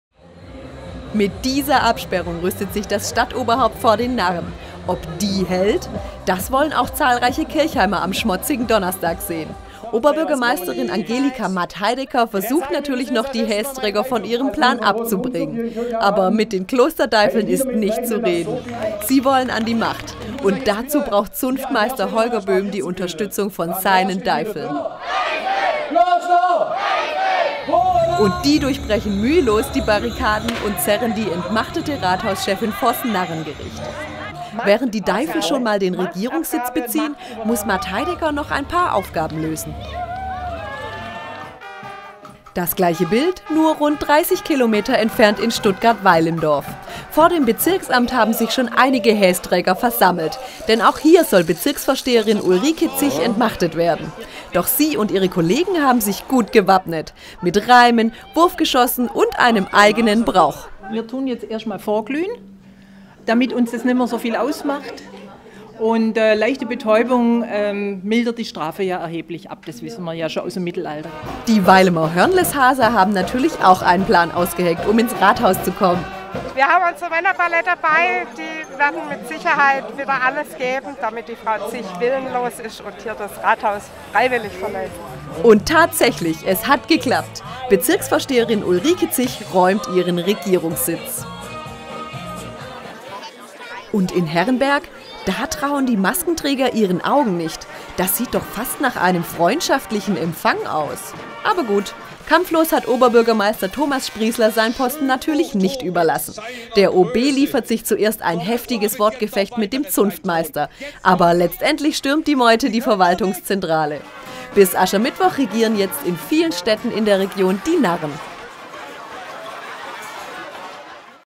In der Region haben jetzt die Narren das Sagen | 09.02.18
Rathäuser wurden gestürmt, Bürgermeister abgeführt und jetzt regieren grässliche Gestalten in den Städten. Alles so geschehen gestern Abend. Bis Aschermittwoch herrschen die Narren in vielen Städten und Bezirken in der Region. Wir waren bei den Rathausstürmen in Kirchheim, Stuttgart-Weilimdorf und Herrenberg mit dabei!